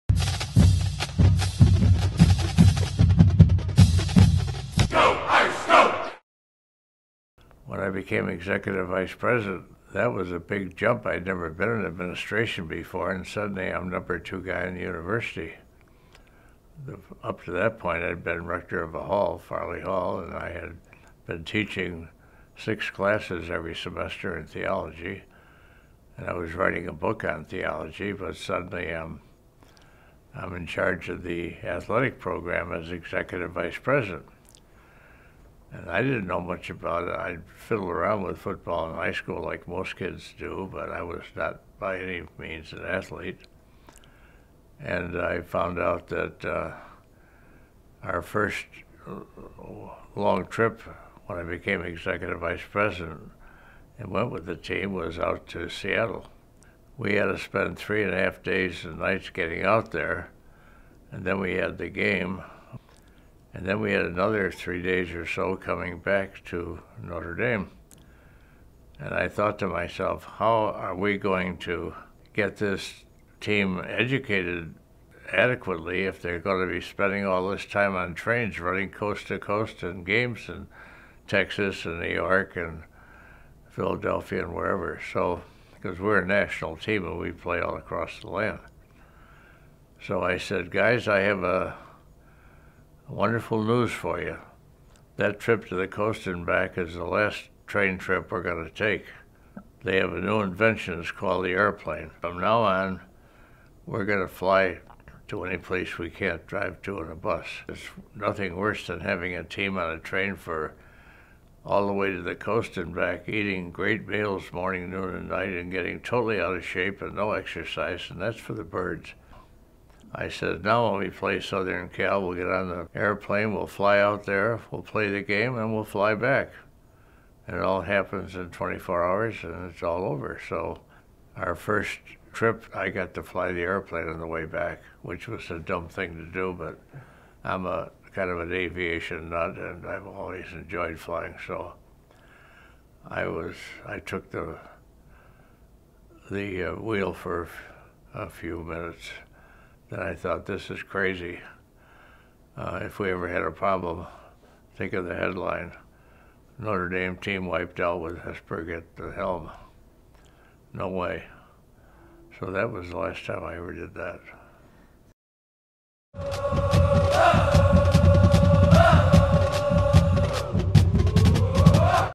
Hesburgh: From Trains to Planes - 125 Years of Notre Dame Football - Moment #001
As part of the celebration of the 125th year of football at Notre Dame, Rev. Theodore Hesburgh, CSC, the President Emeritus of the University of Notre Dame, recalls when the Notre Dame football team changed from trains to planes to travel across the country for their competitions. For more on the history of Irish train and plane travel, visit 125.nd.edu/moments/hesburgh-travel/